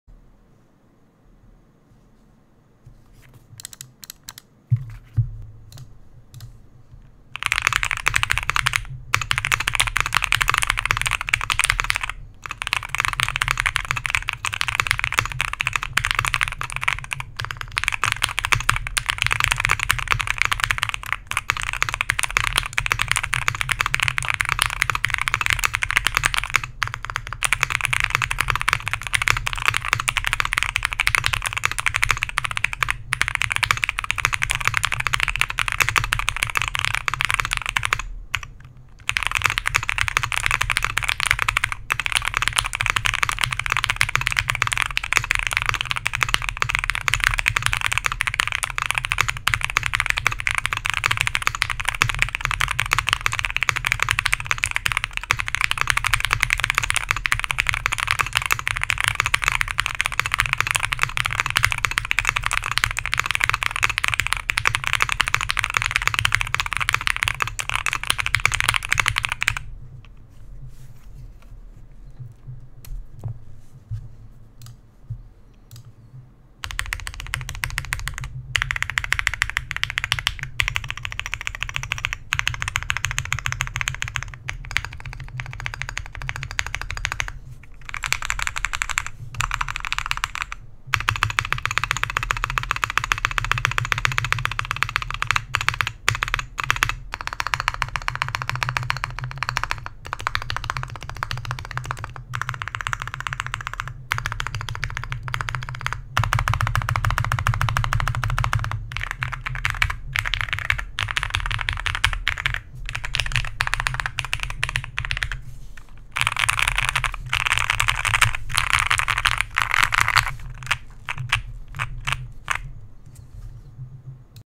Mr. Suit w/ Alpacas on POM Plate
- Keyboard: Navy Mr Suit. WKL
- Plate: POM
- Mounting style: Gasket socks
- Foams: yes PE, yes plate, no case
F13 key: Gold colored Mr. Suit Artisan Stainless Steel

- Switches: Alpacas, lubed and filmed with 205g0 and Deskeys
- Stabilizers: Owlabs Liquid Metal Stabs, 205g0
- Deskmat: GMK Mizu

Thank you towards to my trade partner for trading his WKL for my WK and for gifting me a GMK Mizu deskmat. And thank you to TimKeyLess for building this on stream. He built this with only the PE foam and the sound seemed a little lacking so I asked him to put just the plate foam in and in my opinion, it sounds better.